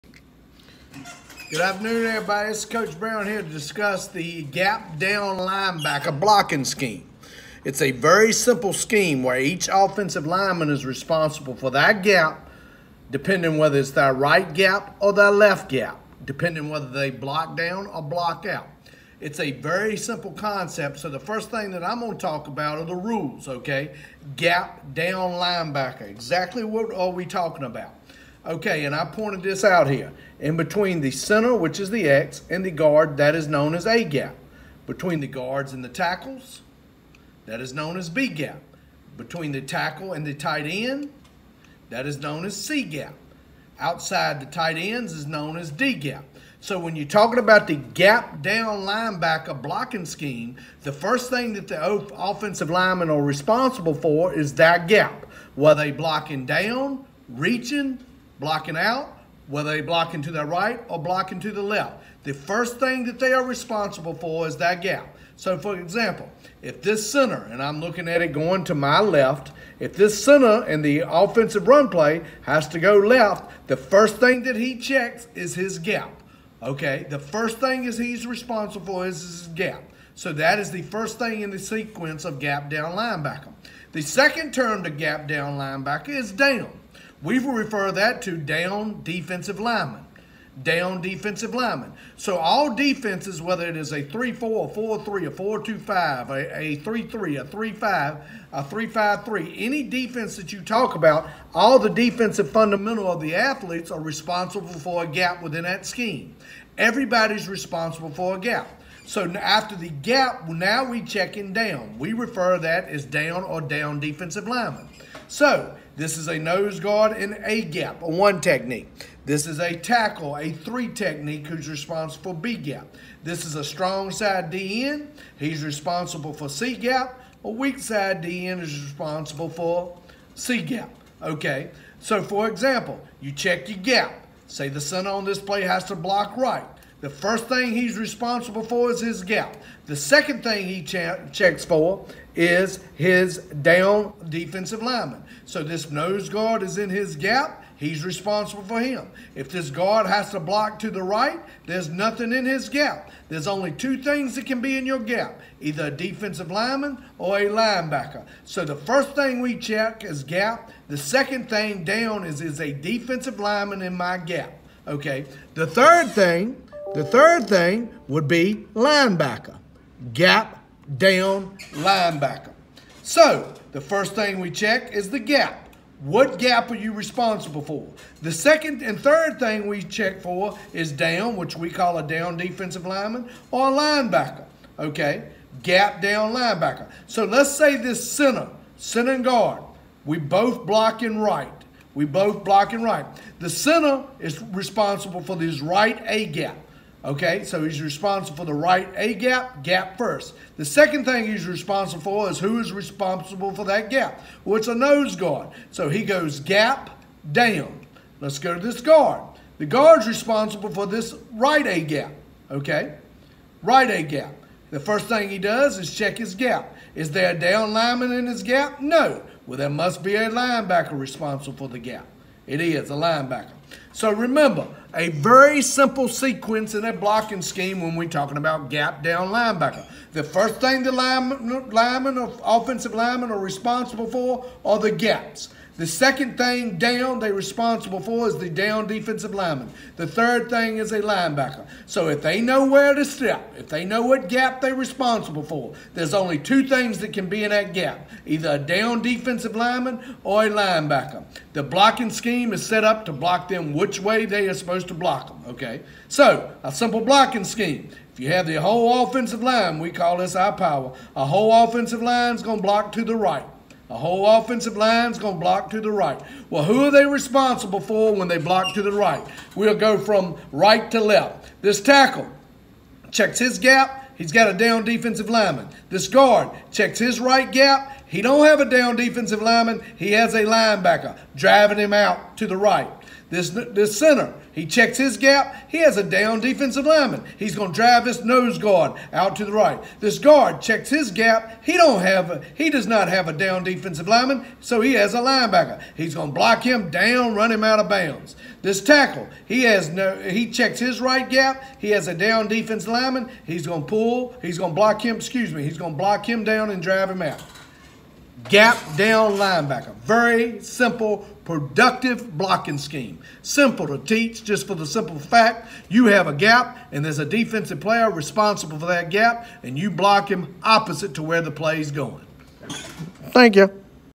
Gap-Down-LB Blocking Scheme
A video that describes each phase of the Gap Down LB blocking scheme.